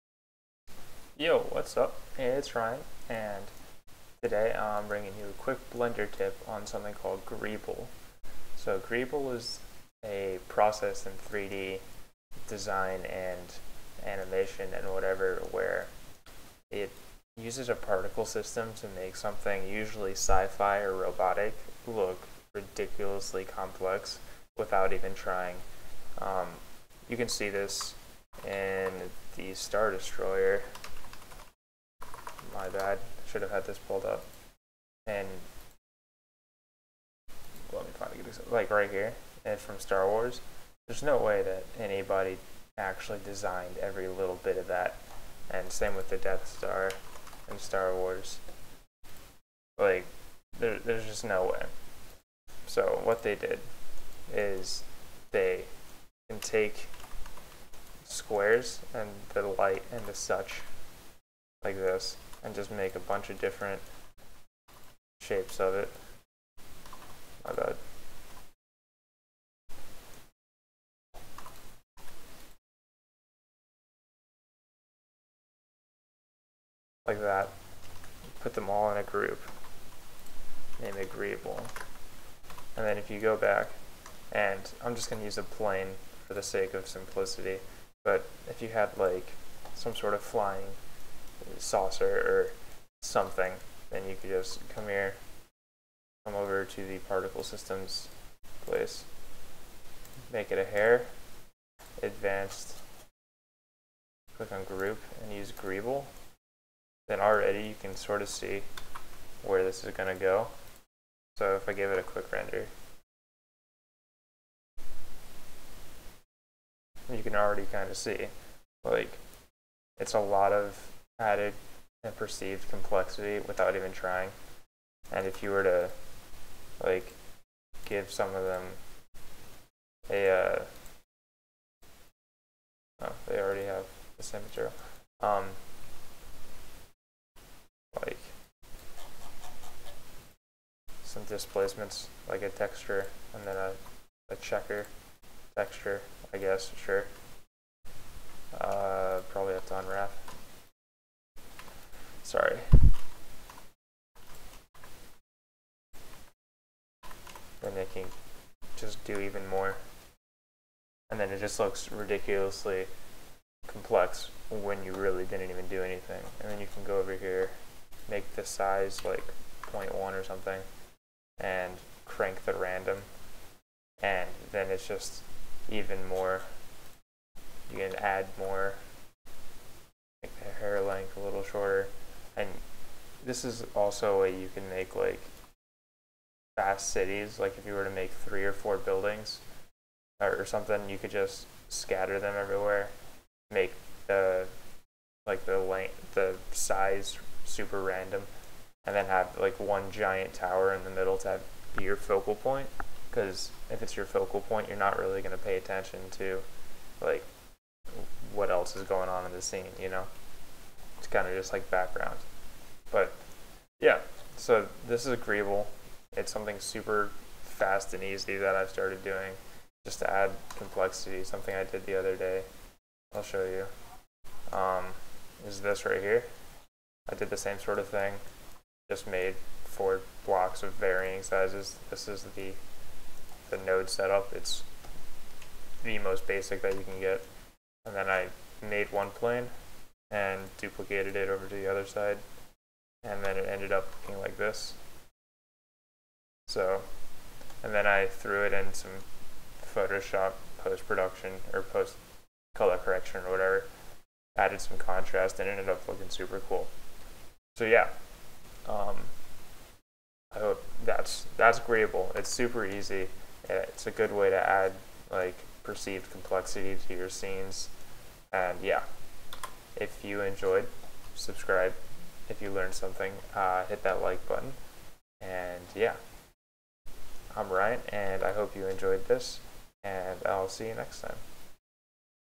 BLENDER QUICK TIP: Greeble
A quick tip on a method that film makers (usually sci fi) will use to add a perceived complexity to their items in scenes.